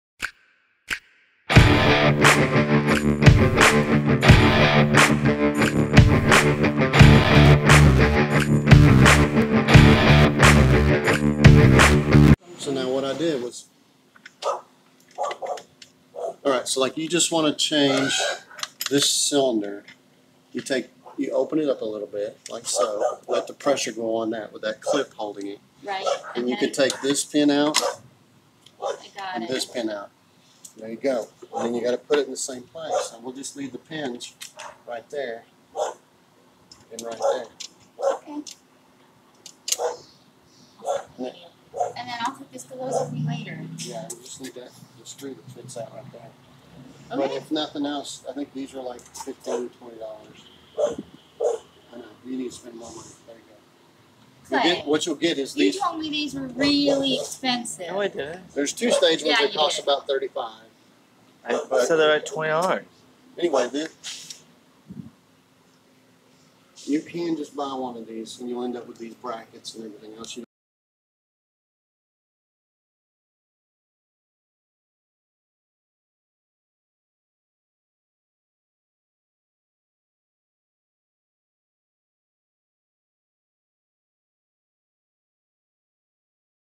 Changing Screen Door Cylinder
Phil walks you through changing those pesky screen door cylinders, all is less than one min.

⏱️⏱️Chapters⏱️⏱️
00:00 Follow along with a handyman

01:23 Be an American not American't